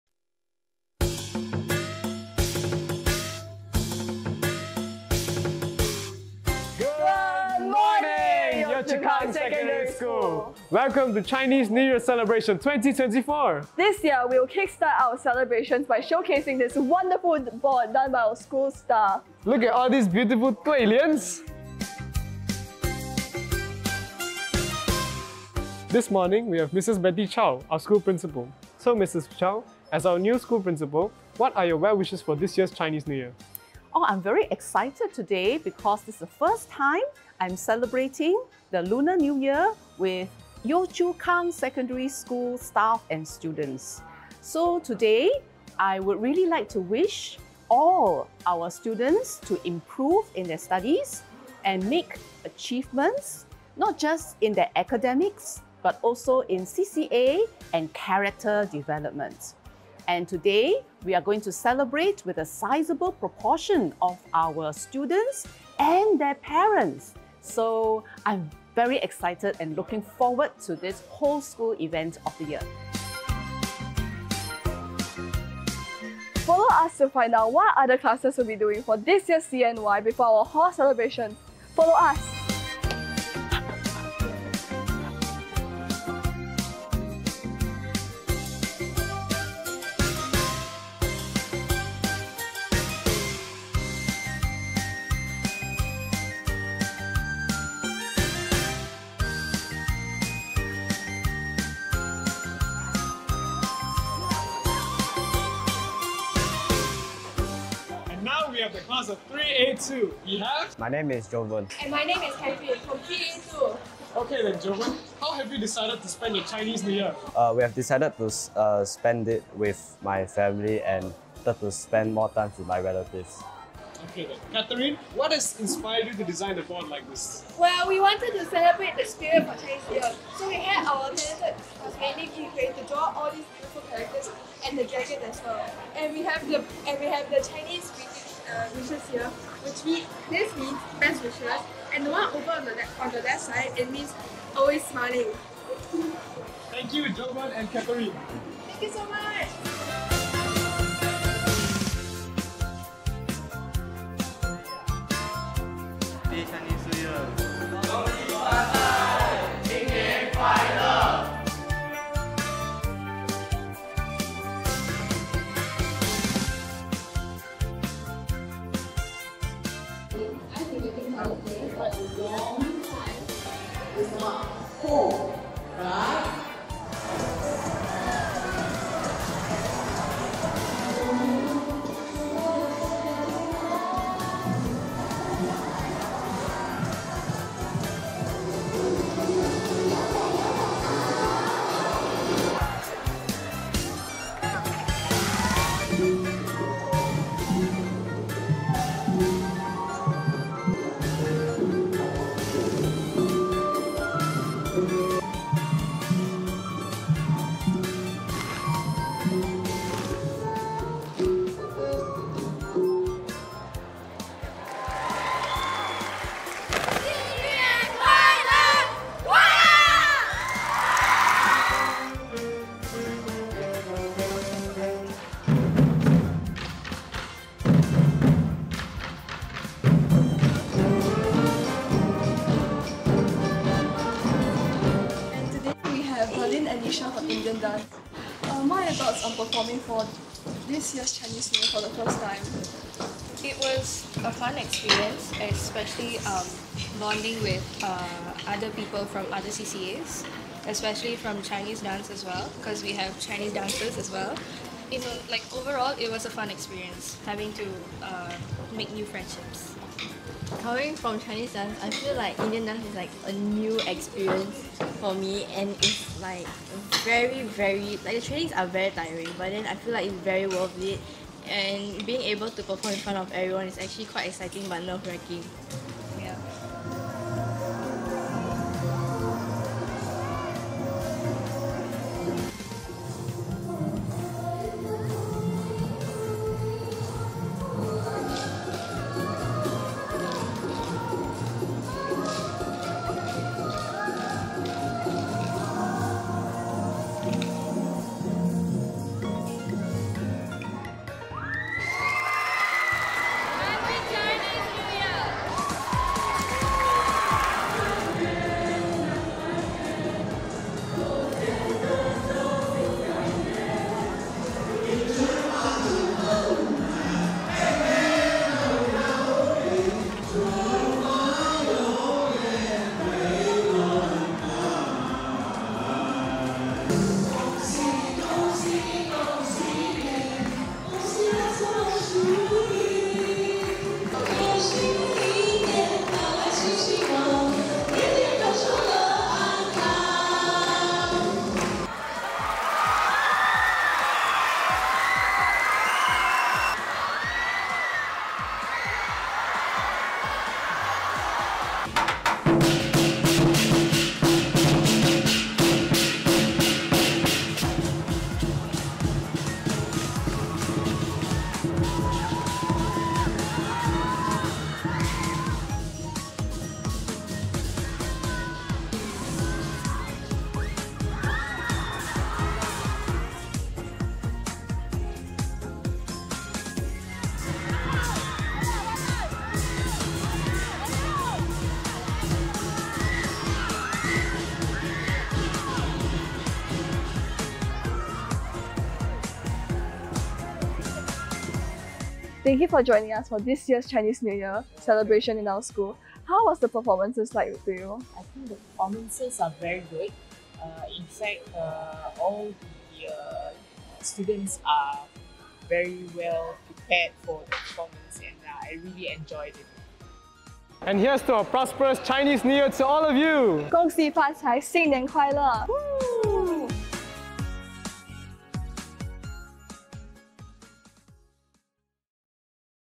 YCKSS CNY 2024 Highlights
Highlights of our Chinese New Year's celebrations on 9 February 2024.

Video taken by: Ethan, Jingyan (Media Club) & Glenda Quek